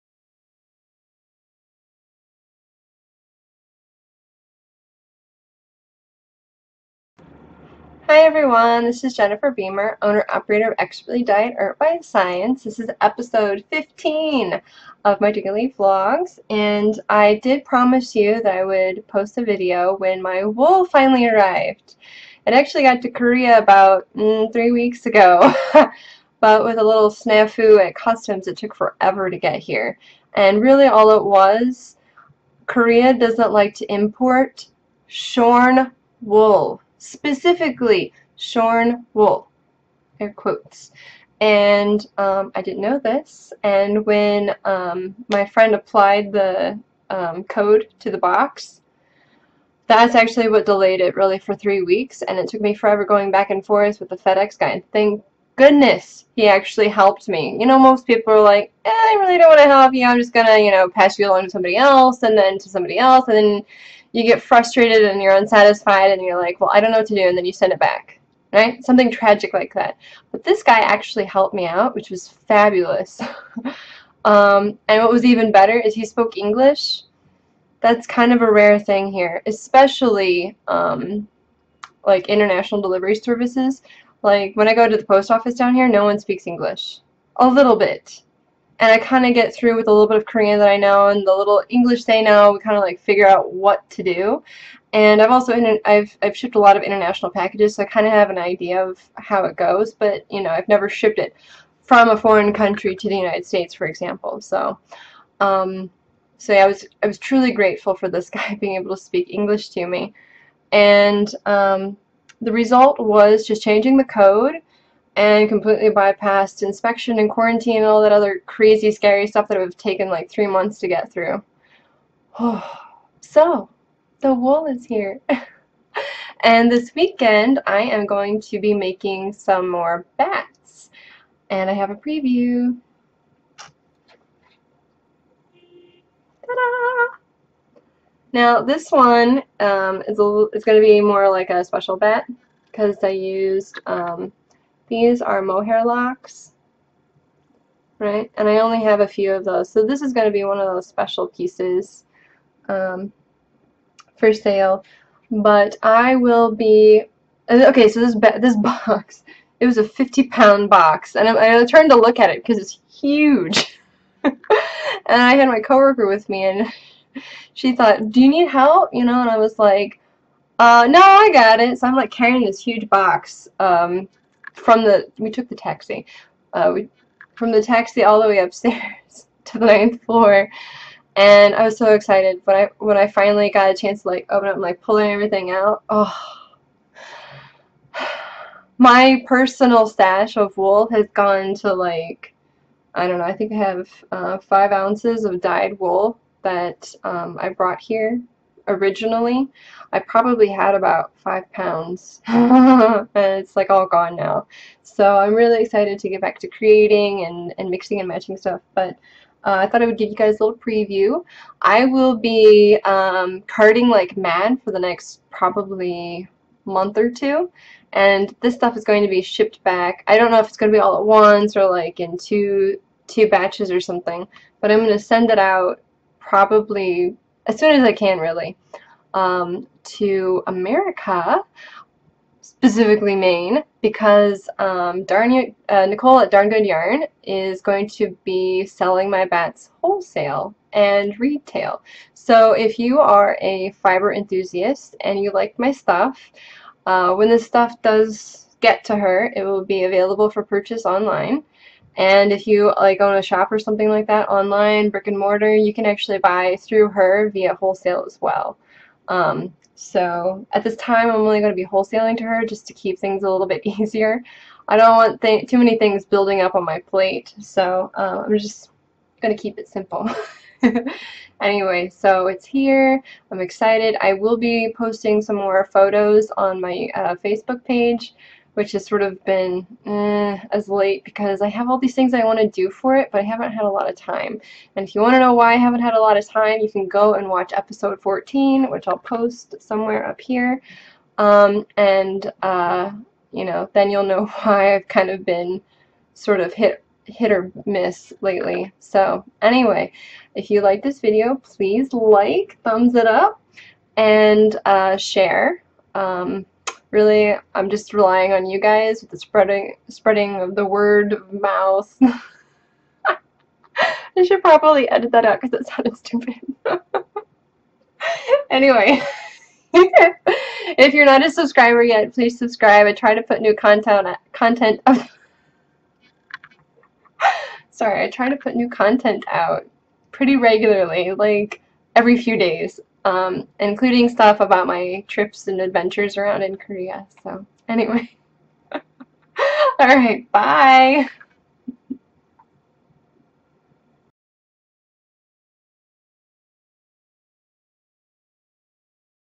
My Wholesale Wool is Here! Thanks FedEx Guy! - Episode 15 - Daily Vlog - Expertly Dyed
I'm back again with Episode 15, where I talk about my wool delivery, and about how long it'll take me to work through 50 pounds of wool. Yeah, that's a huge box. Also, I am sending a huge THANK YOU to the FedEx guy in Seoul who helped me over the 2 weeks I was trying to get my wool out of customs.

Also, did you notice the awesome post-production stuff? Yeah, hubby made the awesome title and end cards for me--my logo looks super fabulous. I'm so glad he's around to help out!